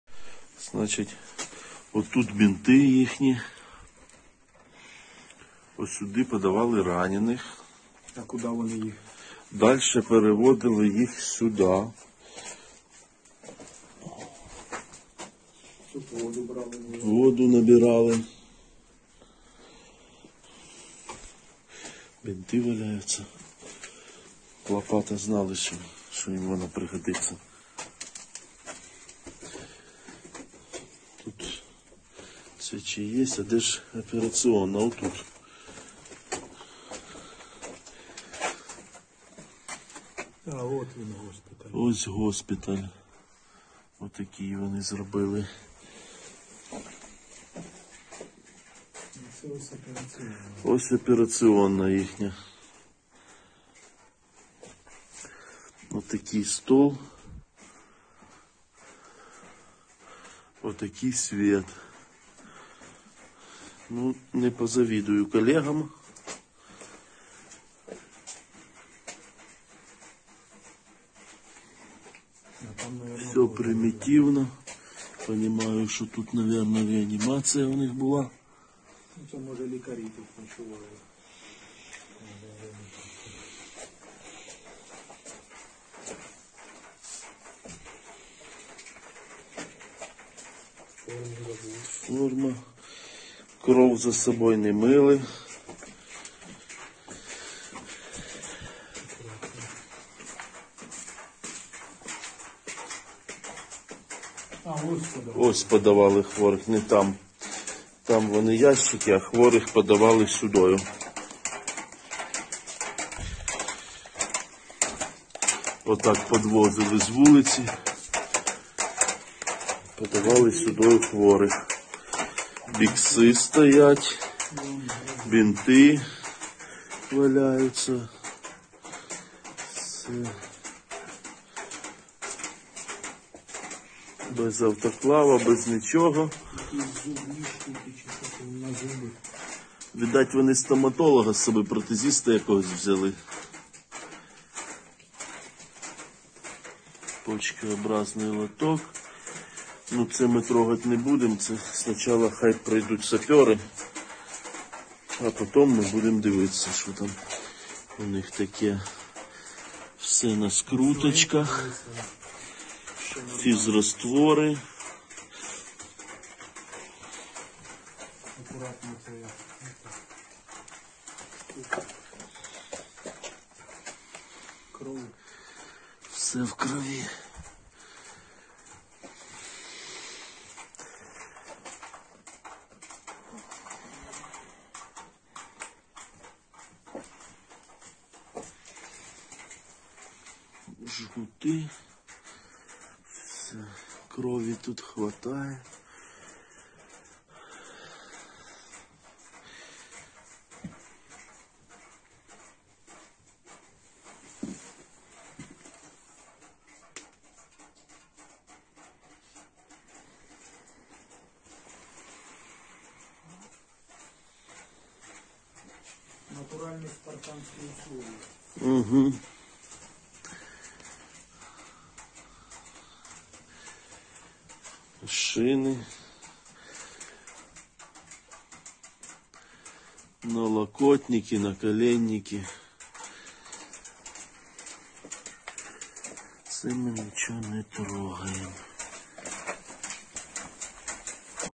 Польовий госпіталь «другої армії світу»
Всі ми знаємо як на полях бою вмирали російські окупанти від ударів нашої артилерії, Javelinov, Стугн.

Але мало хто бачив у яких умовах помирали ті росіяни, хто залишився живим, але був поранений і направлений у польовий, з дозволу сказати, госпіталь.

Сьогодні мені надіслали раніше не опубліковане відео із селища Нова Басань, Чернігівської області, де орки розгорнули військово-польовий шпиталь.

Ось так ви будете здихати в брудному підвалі повному антисанітарії без мінімальних людських умов та шансів на одужання!